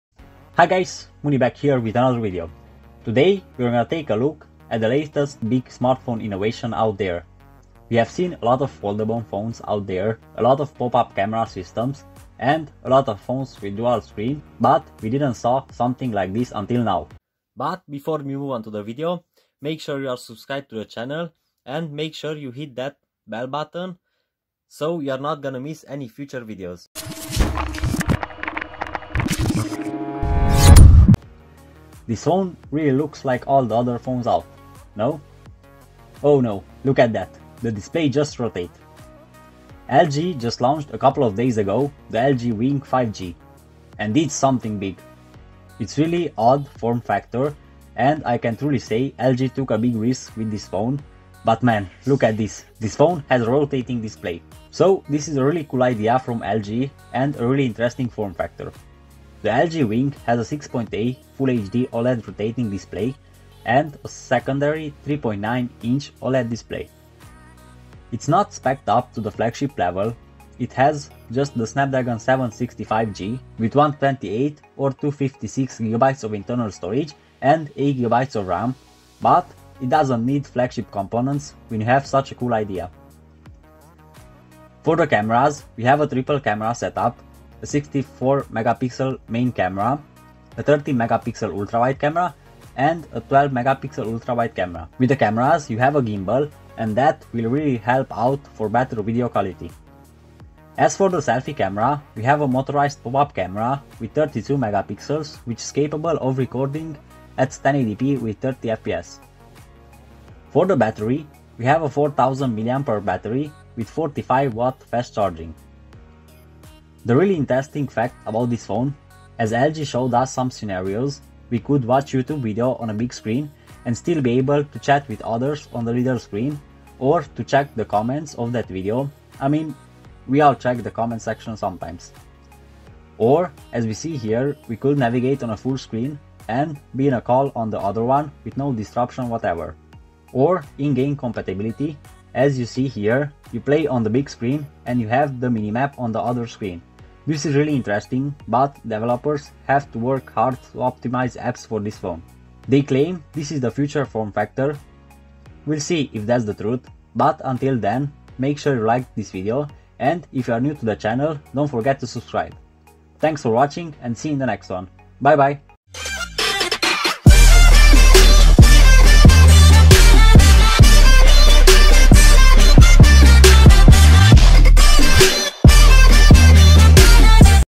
LG WING 5G FIRST IMPRESSIONS!
Today we take a look at the new LG WING!